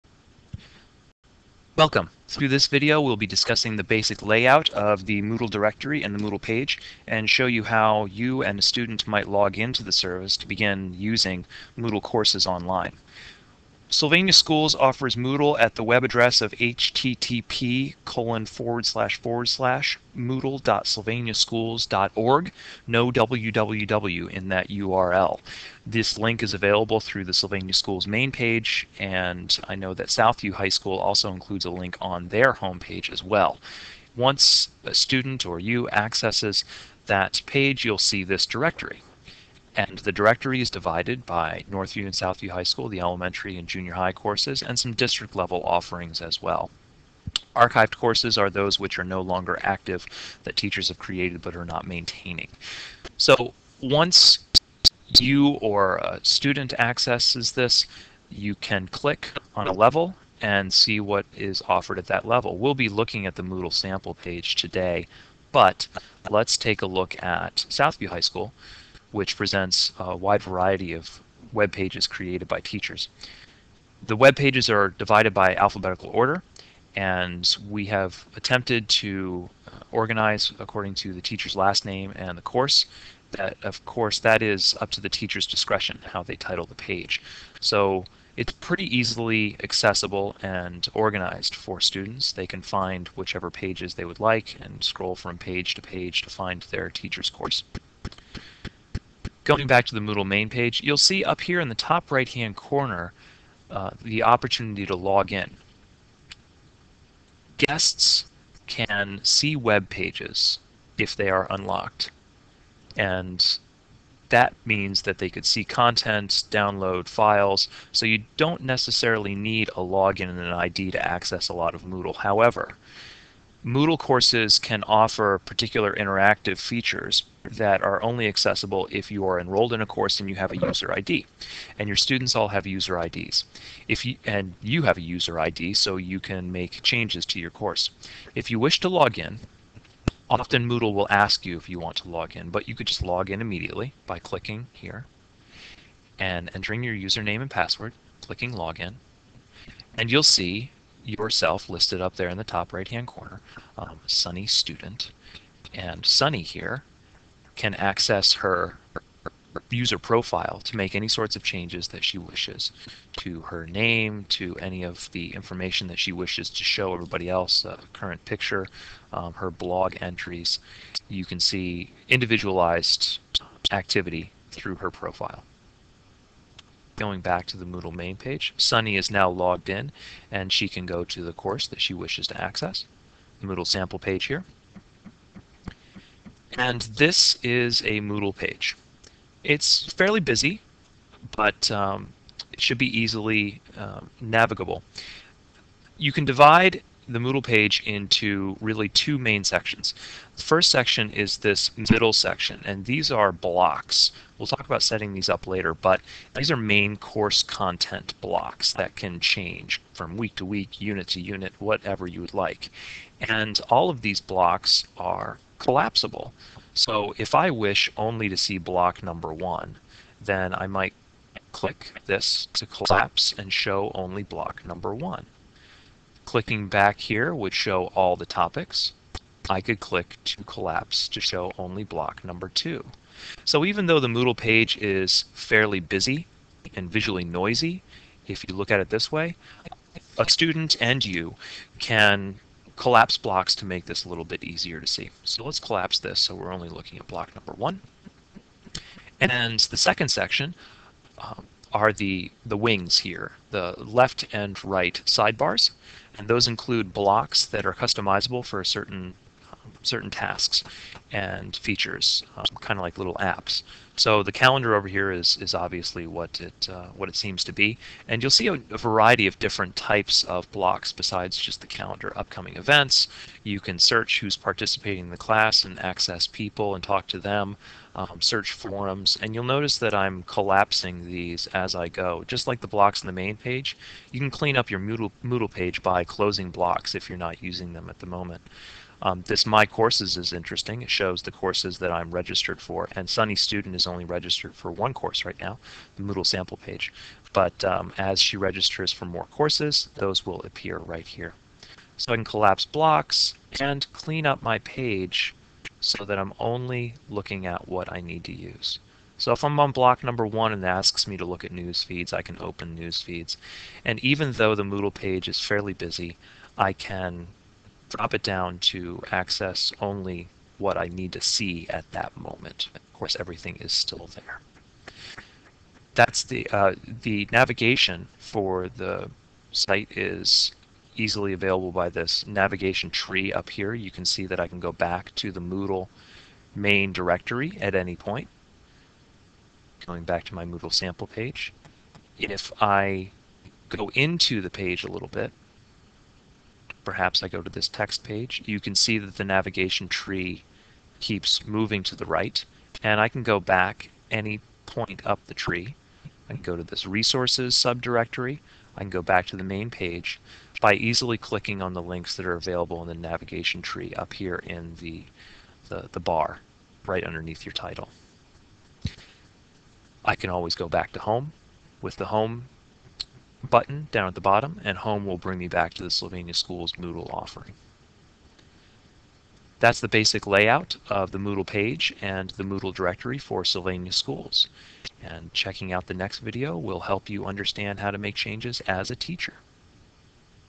Moodle Tutorial 1: Basic Layout
These videos are designed for Sylvania Schools teachers wishing to develop and use Moodle pages for their classes.  This first video discusses the basic layout of the Moodle directory and Moodle page.  It also explains how to log in as a unique user.